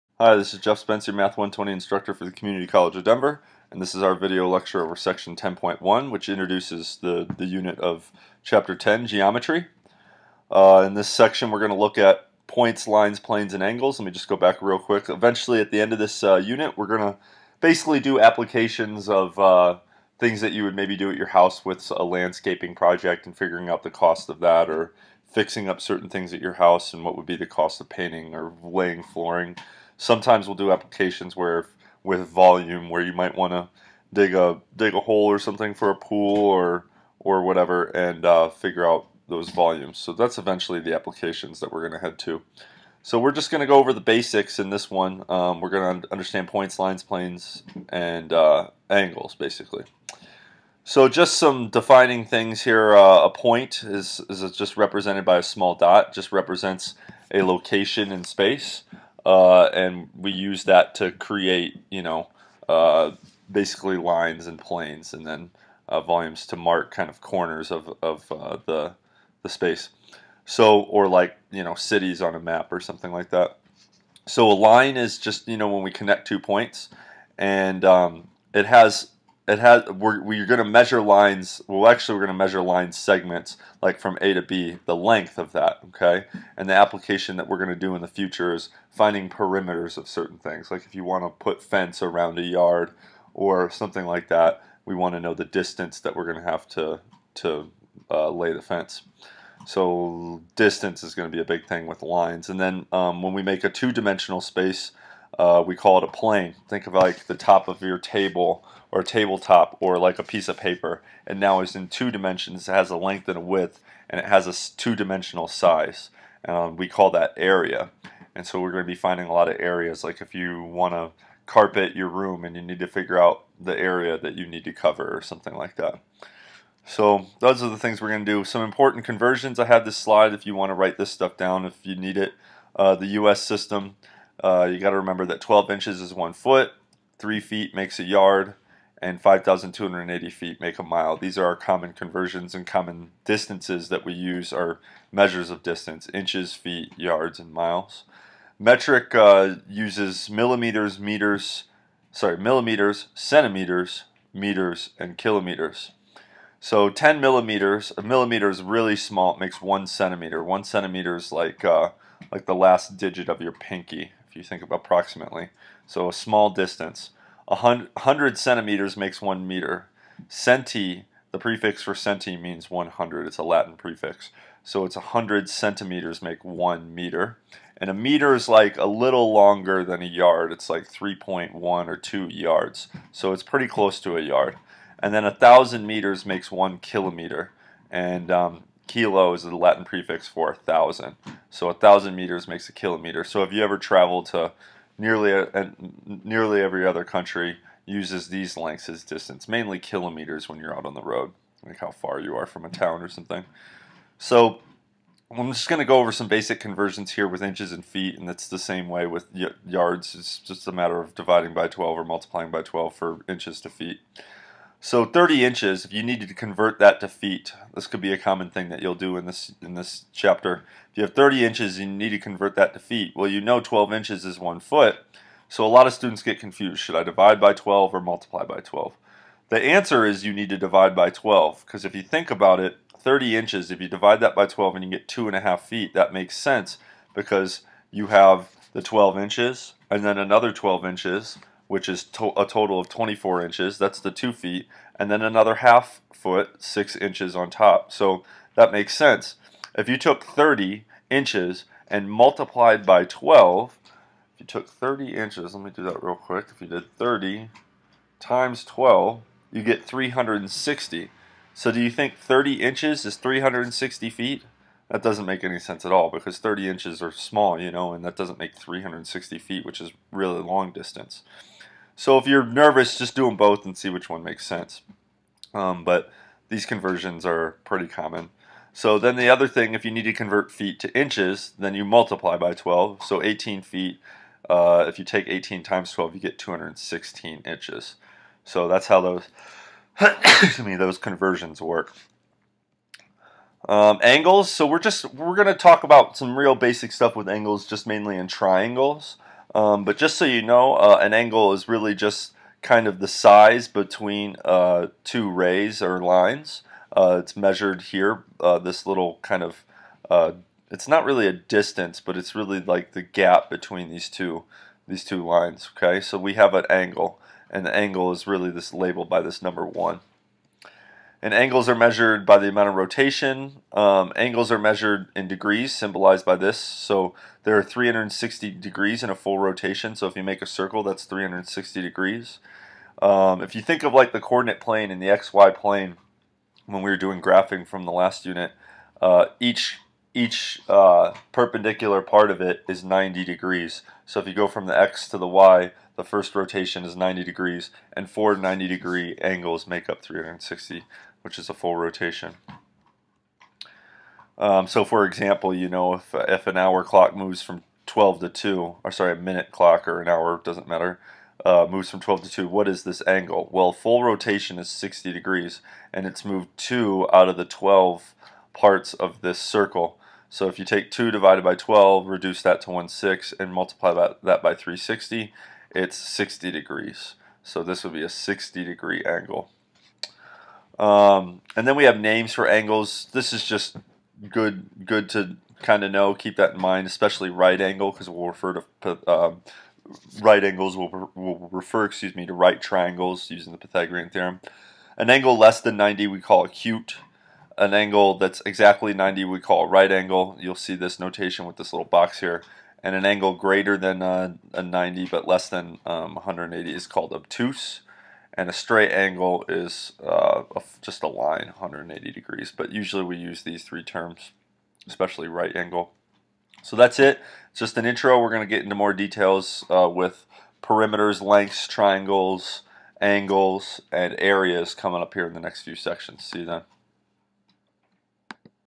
10.1 (120 Blitzer)
Table of Contents:

00:00 - CHAPTER 10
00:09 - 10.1
00:13 - CHAPTER 10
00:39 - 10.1
00:41 -
00:49 - Defining Points, Lines and Planes
02:07 - Length Conversions
04:52 - Angles
05:20 - Measuring Angles Using Degrees
05:53 - Example: Using Degree Measure
06:20 - Classifying Angles by Their Degree Measurement